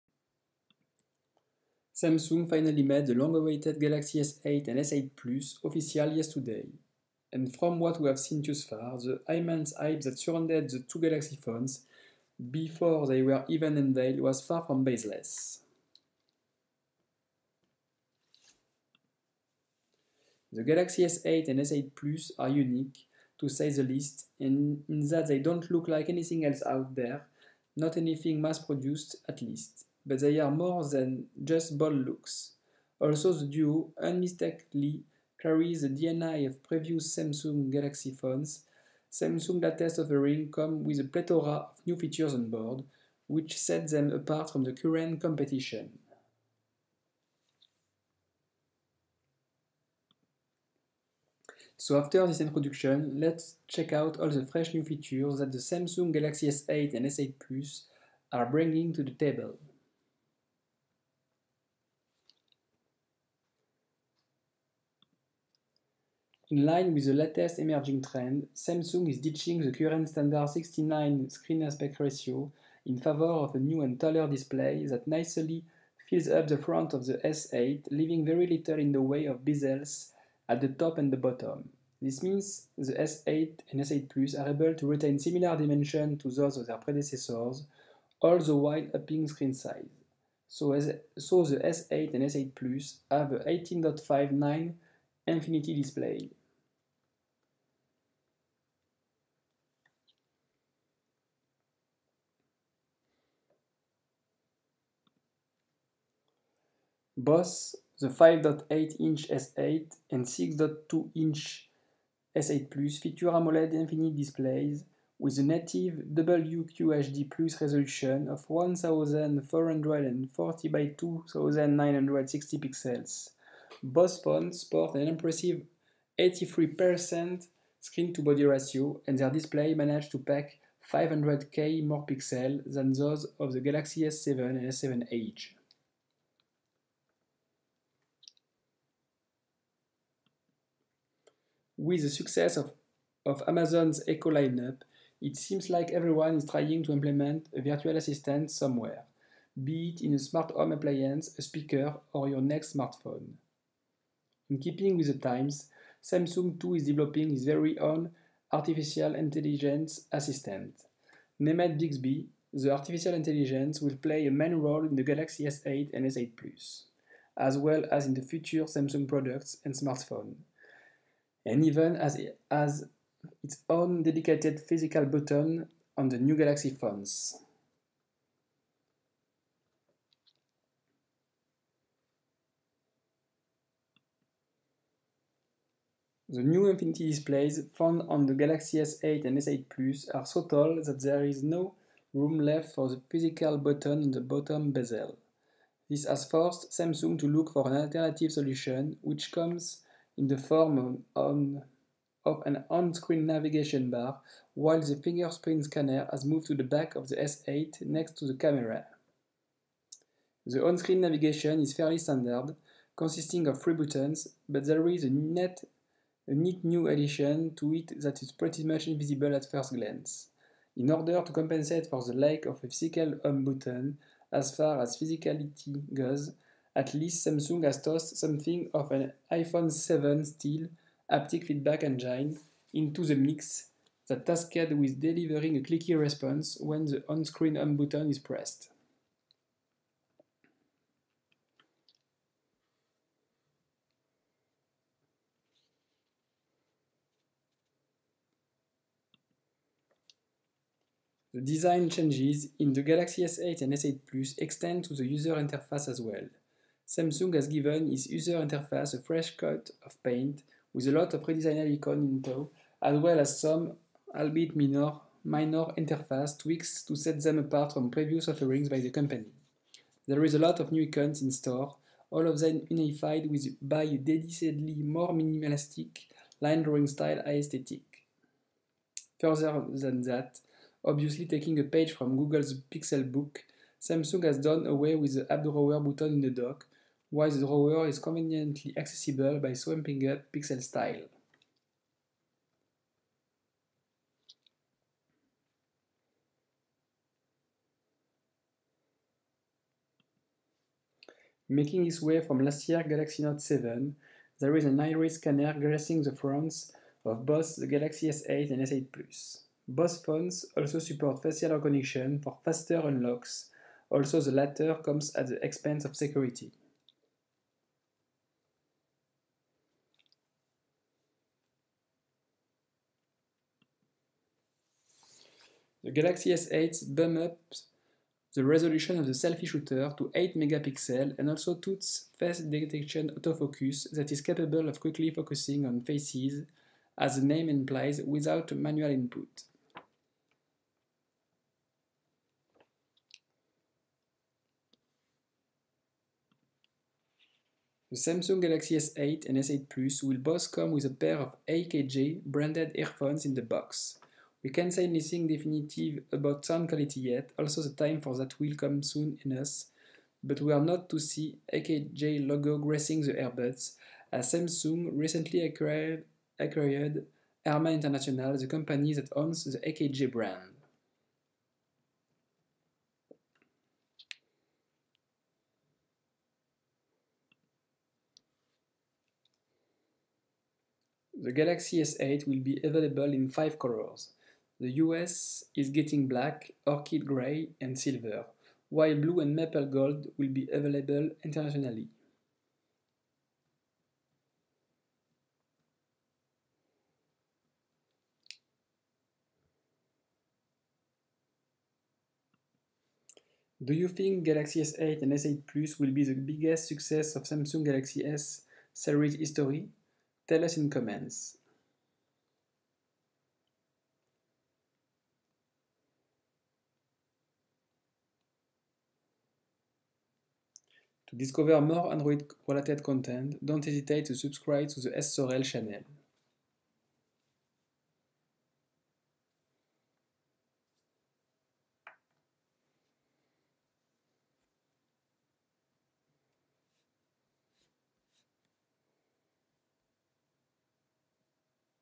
Discover new features of the Samsung Galaxy S8 / S8+
Discover the new features of the Samsung Galaxy S8 / S8+ in depth with that video. The video covers the Infinite Display, Bixby, the Iris Scanner and all the new exciting features of the Galaxy S8 and S8+